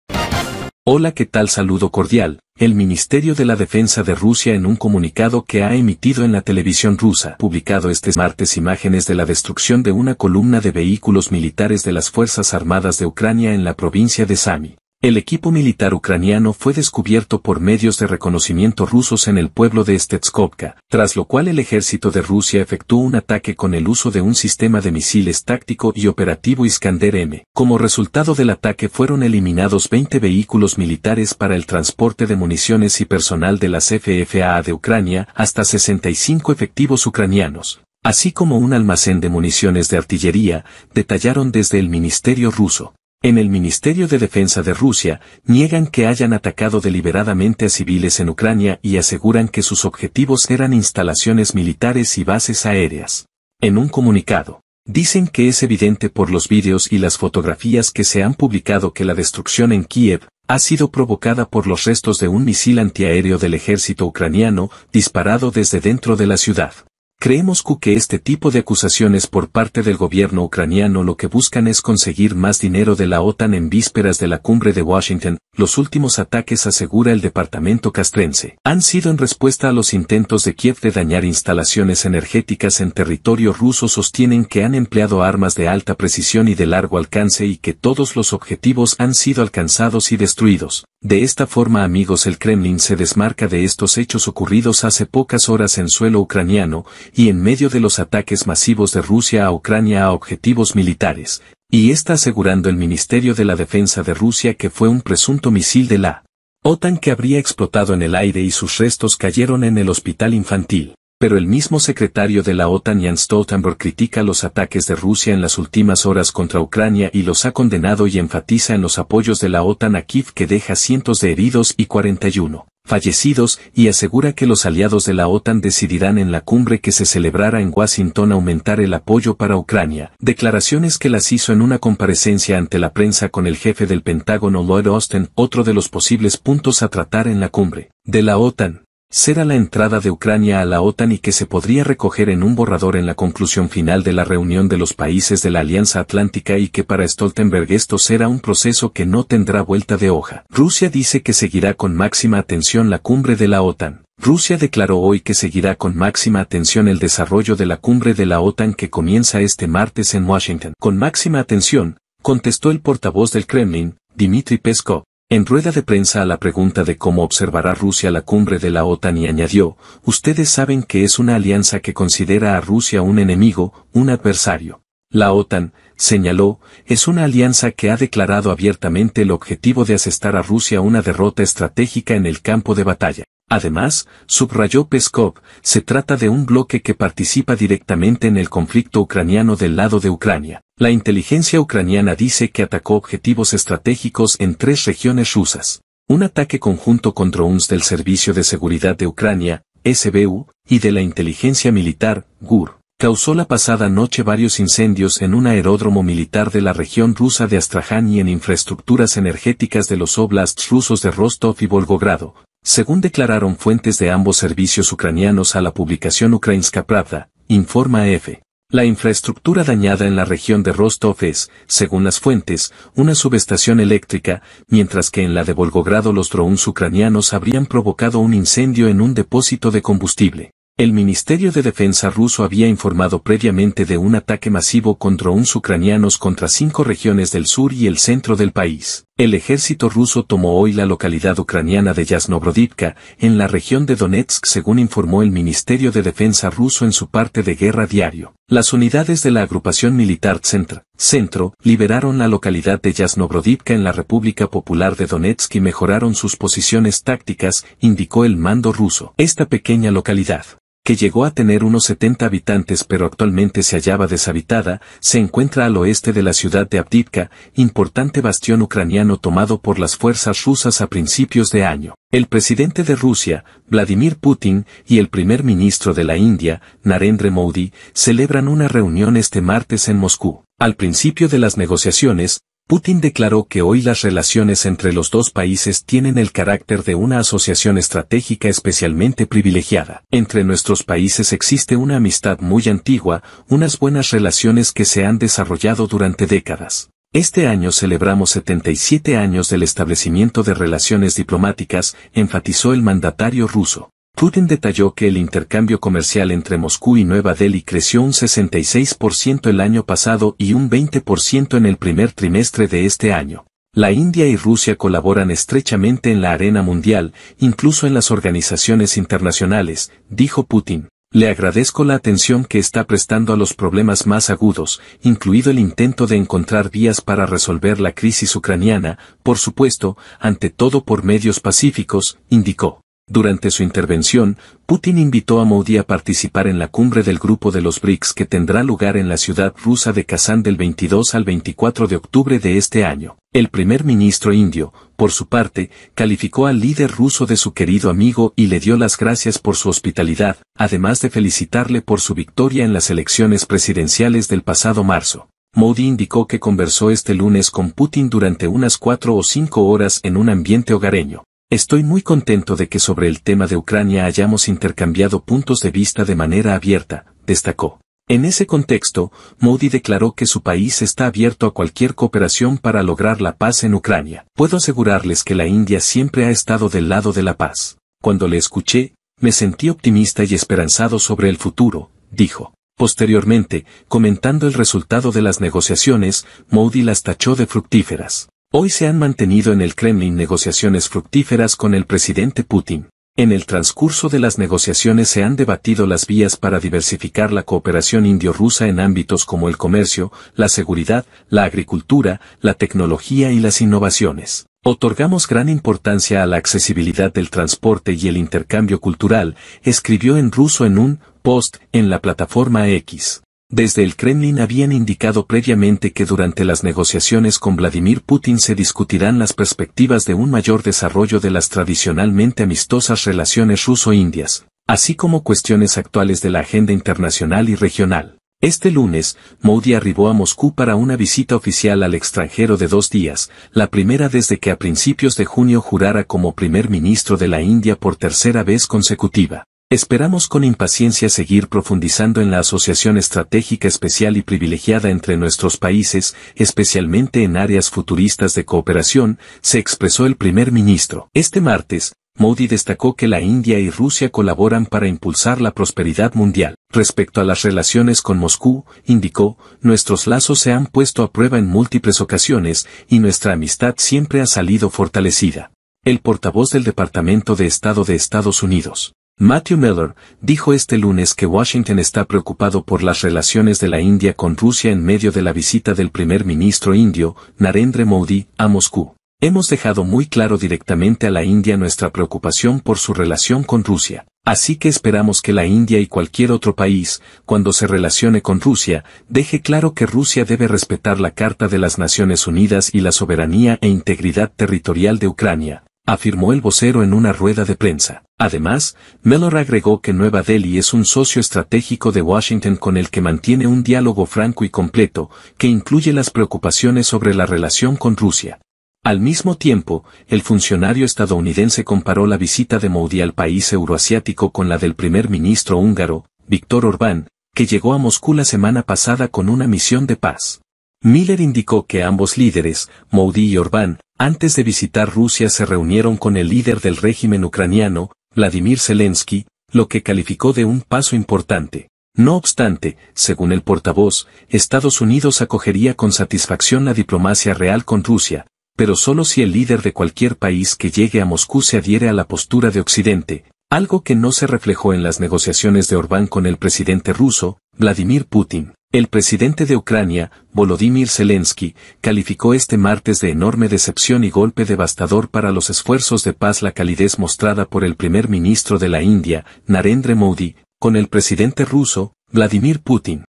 ARTILLERIA RUSA DESTRUYE COLUMNA BLINDADOS PUTIN Y MODI PACTAN EEUU INCOMODO POR ALIANZA
🚨Boletin de Noticias internacionales de hoy 09de JULIO de 2024.